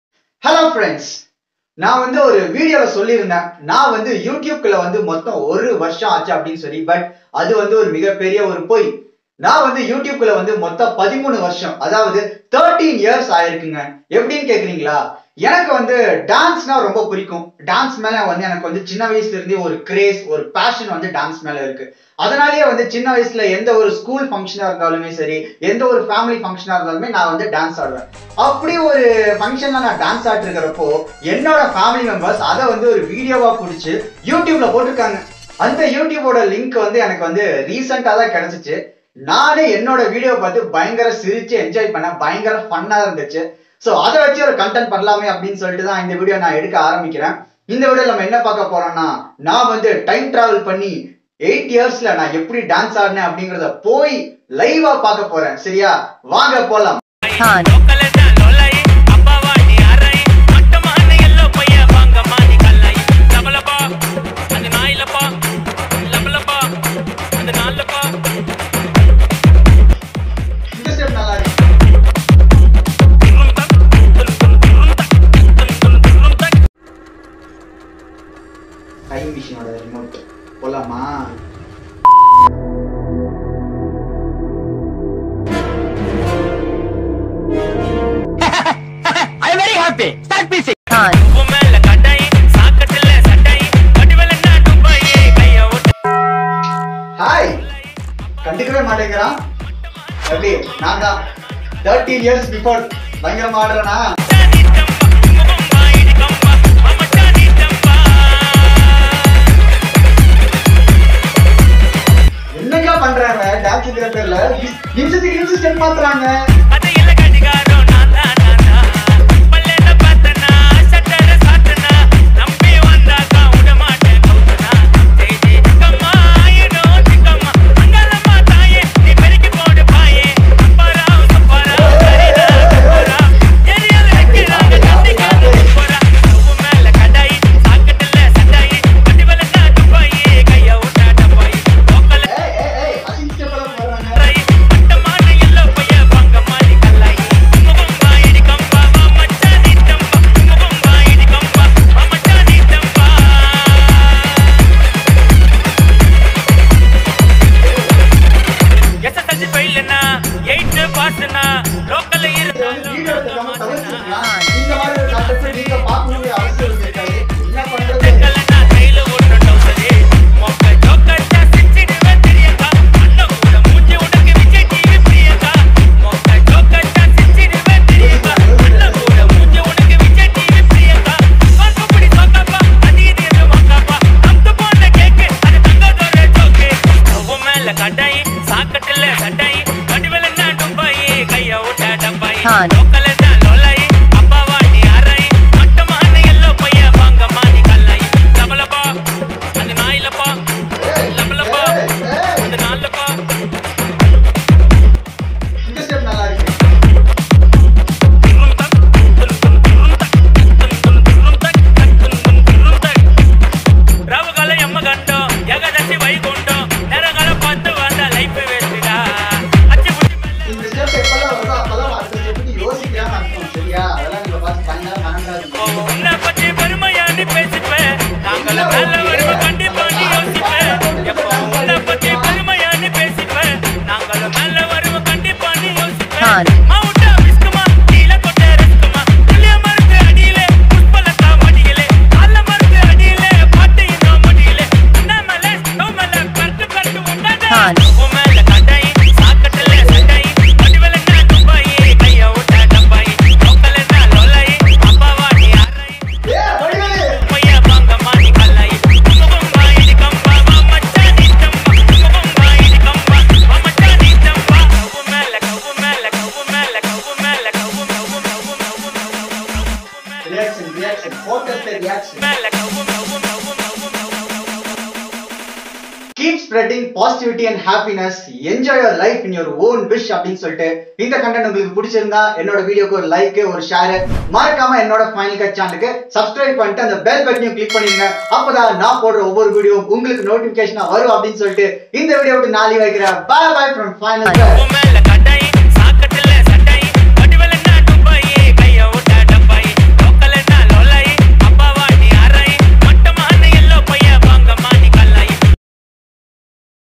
Time Travelling to 2008|Special Funny Dance Moment😂😂😂|feat.Gokul Kannan
Hi friends,this is a video about my dance video got uploaded on youtube 13years ago!!!Its just a funny try on how i react to it live by time travelling to 2008.